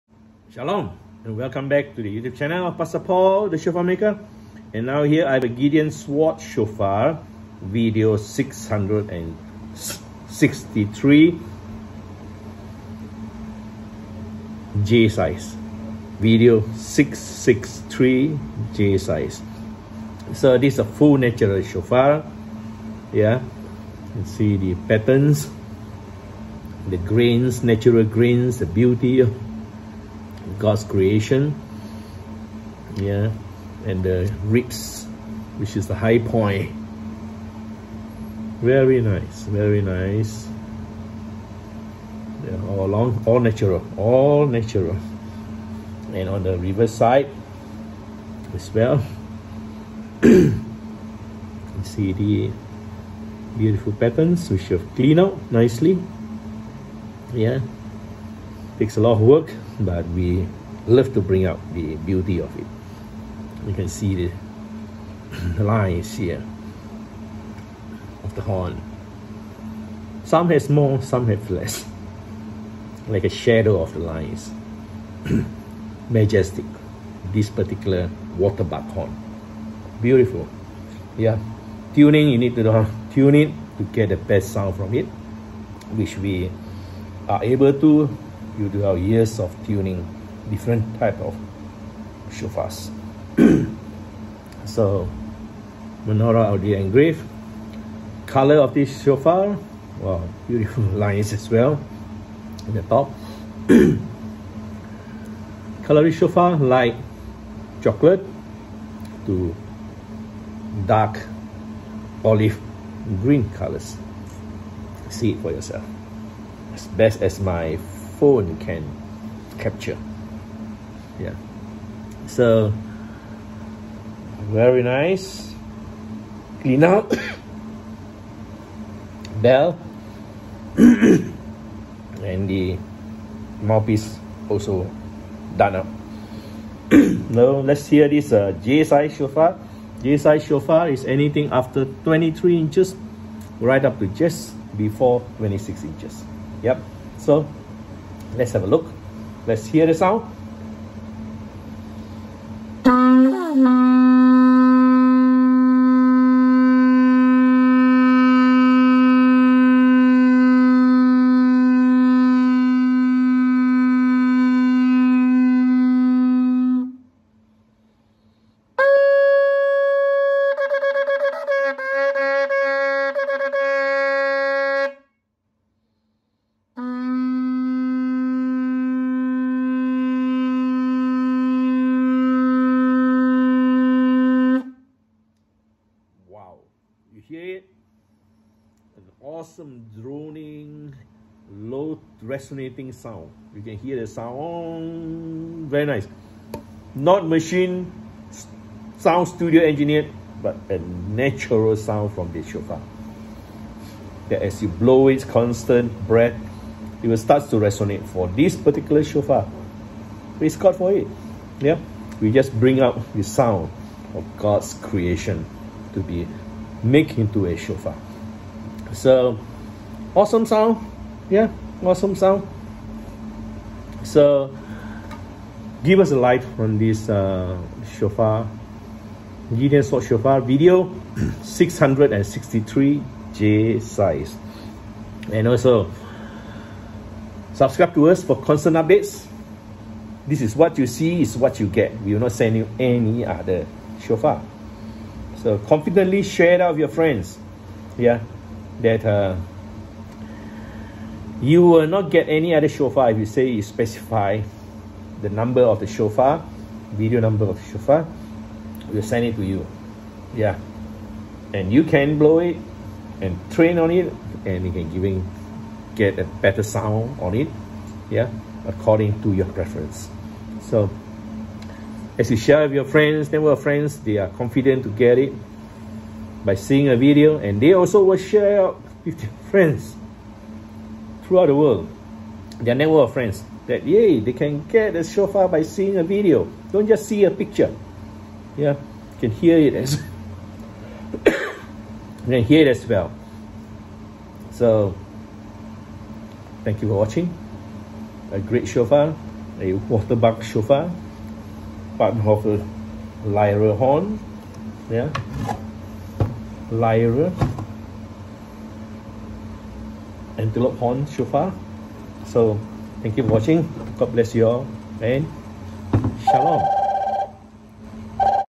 SOLD •ShofarMaker v663_J. Gideon Sword Shofar. Full Natural.
A Droning Resonating Awesome Low to High Pitch Double Tone Fine Tuned Shofar.
This Shofar is also termed as a Lyre Antelope Horn.
Full Natural Finish Shofar passionately crafted to exposed the natural grains and keep the intricate Ribs of the magnificently beauty of the horn as created by our Creator God.
Light Chocolate Dark Olive colours.
View & Listen to appreciate the Natural Horn of this Waterbuck Antelope.
my Utmost for His Highest.
Shalom & Ybu.